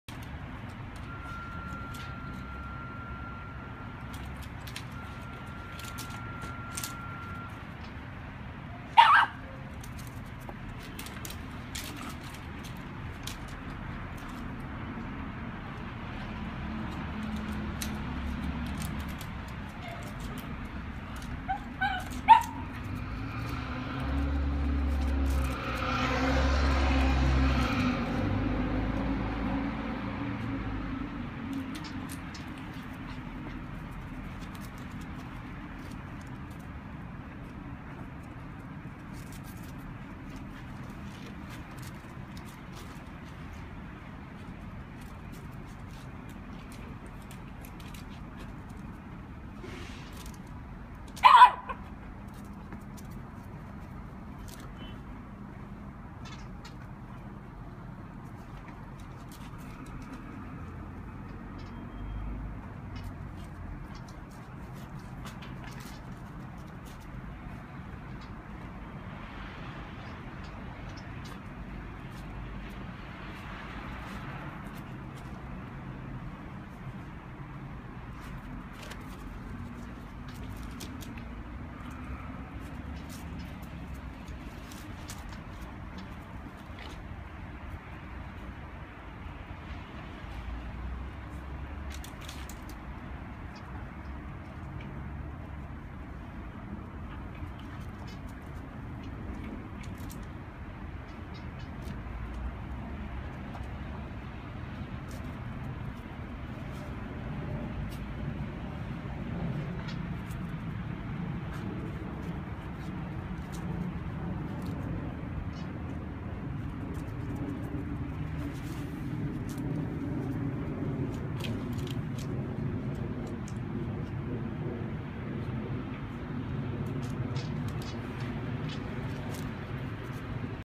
German Shepherd Puppy and Snow Day
Rambo the German Shepherd enjoying the snow when he was a puppy.

Video uploaded by Meet the Chows.